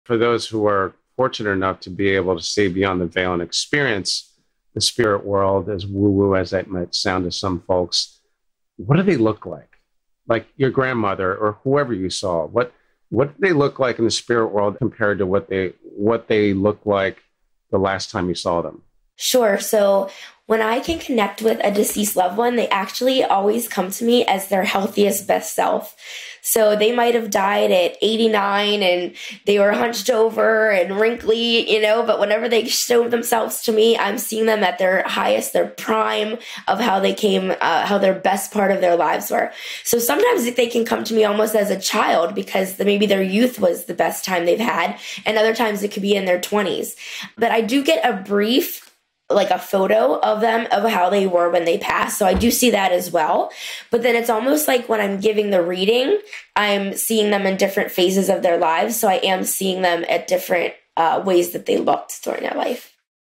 What Do Our Loved Ones Look LIke In The Spirit World?
@brandivanmedium talks about connecting with our loved ones, and what they look like in the spirit world